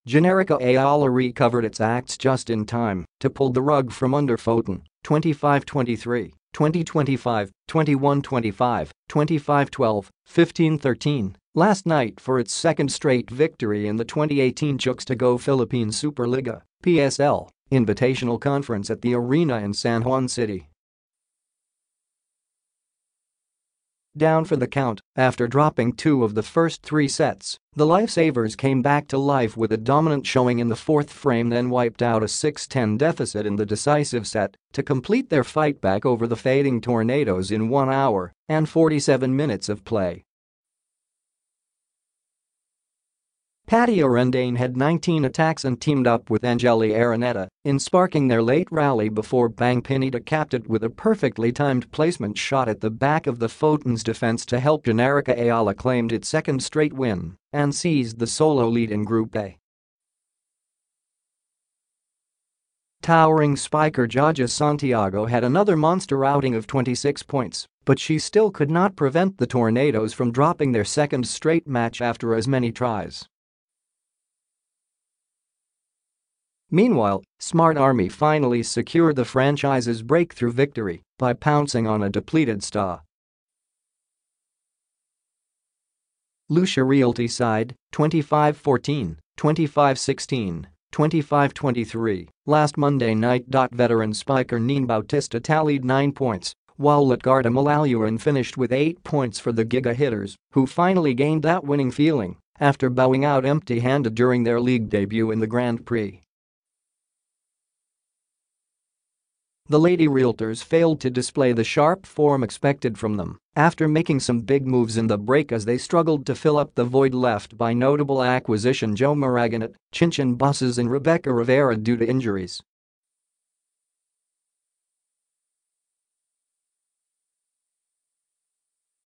PSL: Lifesavers fend off Tornadoes
Generika-Ayala recovered its acts just in time to pull the rug from under Foton, 25-23, 20-25, 21-25, 25-12, 15-13, last night for its second straight victory in the 2018 Chooks to Go-Philippine Superliga (PSL) Invitational Conference at The Arena in San Juan City.

Down for the count after dropping two of the first three sets, the Lifesavers came back to life with a dominant showing in the fourth frame then wiped out a 6-10 deficit in the decisive set to complete their fightback over the fading Tornadoes in one hour and 47 minutes of play.

Patty Orendain had 19 attacks and teamed up with Angeli Araneta in sparking their late rally before Bang Pineda capped it with a perfectly timed placement shot at the back of the Foton’s defense to help Generika-Ayala claimed its second straight win and seized the solo lead in Group A.

Towering spiker Jaja Santiago had another monster outing of 26 points but she still could not prevent the Tornadoes from dropping their second straight match after as many tries.

Meanwhile, Smart-Army finally secured the franchise’s breakthrough victory by pouncing on a depleted Sta. Lucia Realty side, 25-14, 25-16, 25-23, last Monday night.

Veteran spiker Nene Bautista tallied nine points while Lutgarda Malaluan finished with eight points for the Giga Hitters, who finally gained that winning feeling after bowing out empty-handed during their league debut in the Grand Prix.

The Lady Realtors failed to display the sharp form expected from them after making some big moves in the break as they struggled to fill up the void left by notable acquisition Jho Maraguinot, Chinchin Basas and Rebecca Rivera due to injuries.